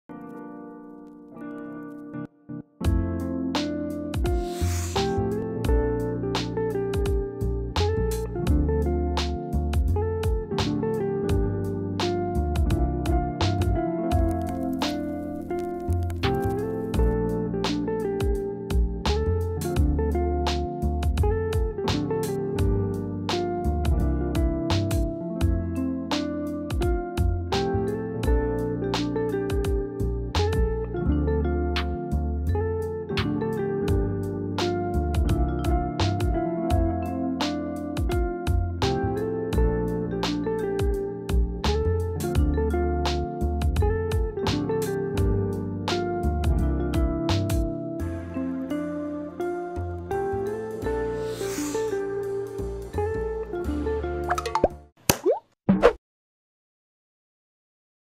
PHILIPS 3 Blades Air Circulation Fan with LED Display | LOVE SHOPPING.PH
📌 SPECIFICATIONS:
CONTROL METHOD: Digital Touch Panel / Remote Control
SWING MODES: Left & Right (Automatic Swing); Up & Down (Manually Adjusted)
AIR SUPPLY DISTANCE: 7 Meter
NOISE: ≤60dB
WIND GEAR SPEED: 3 Level
TIMER SETTING: Up to 12 HOURS
RATED POWER: 45W
RATED FREQUENCY: 50Hz
RATED VOLTAGE: 220V